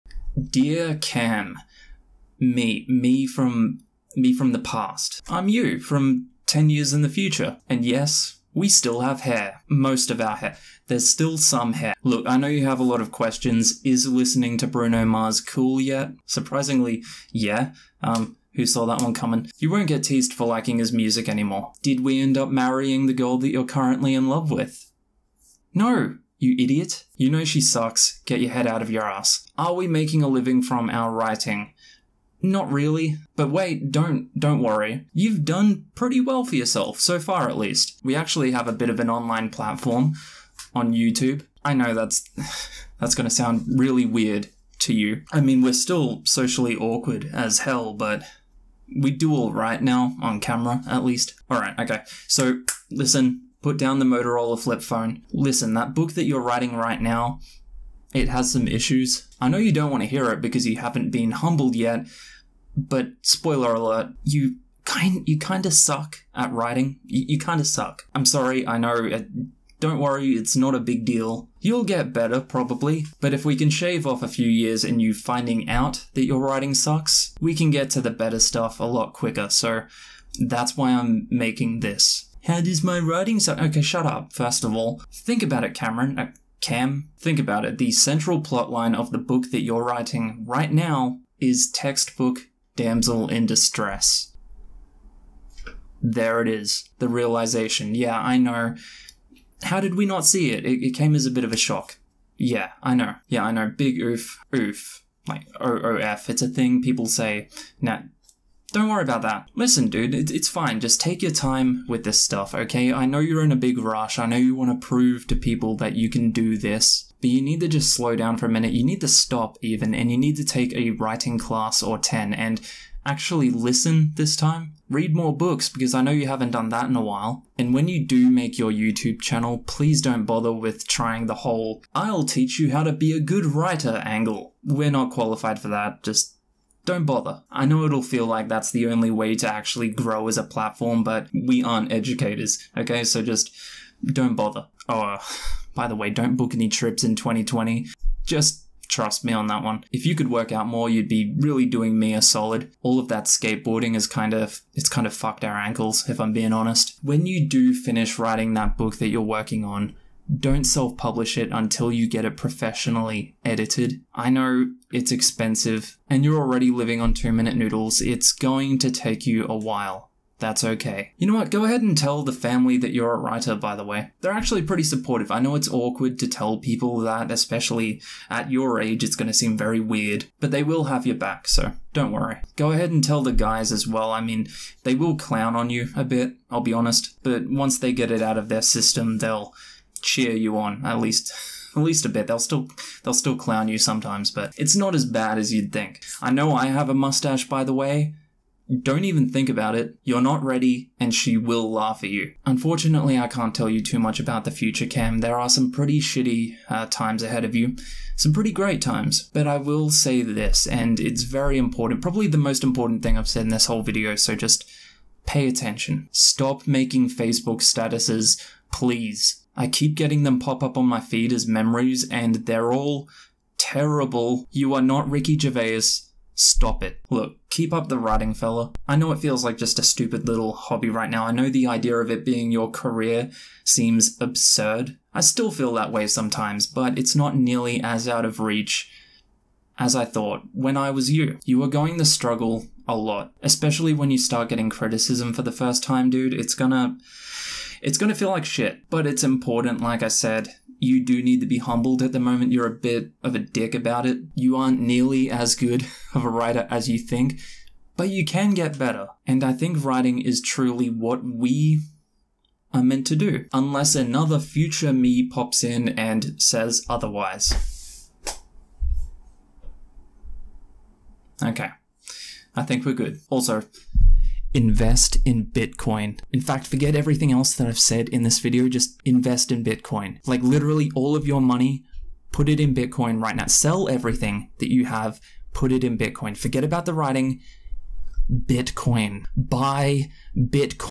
Dear Young Writer (me)
This video is for the me of 10 years ago.

(Open Me)

(Please don't ask me to read/review your book in exchange for money or a free copy)

Videos on writing and the setbacks of being an aspiring Author.